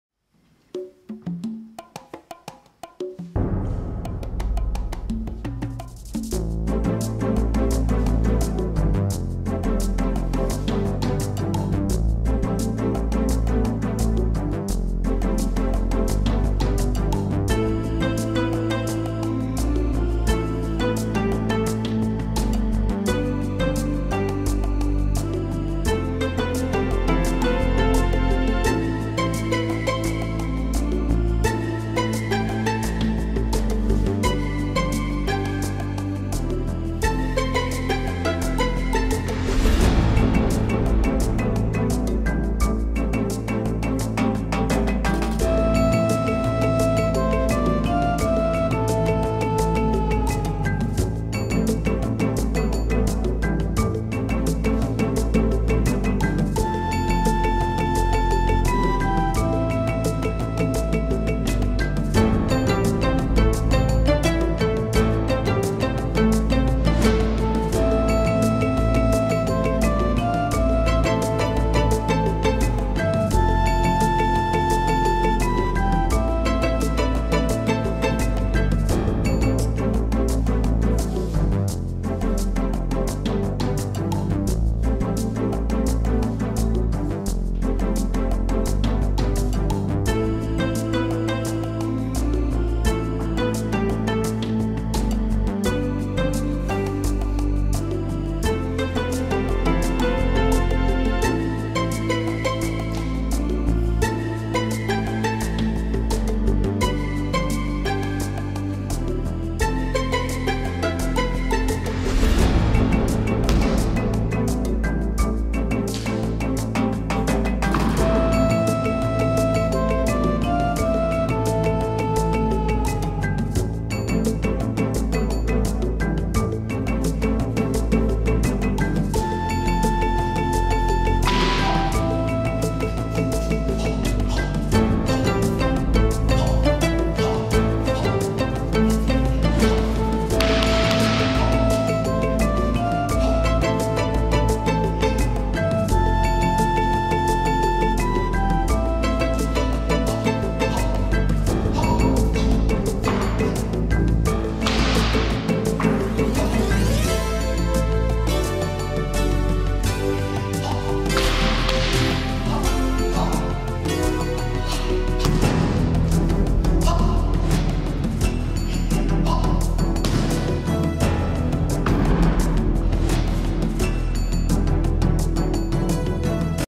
Bob Fermor Pole Form
Guan Yu International Academy of Martial Arts,
 Bob Fermor performing eye brow level pole form.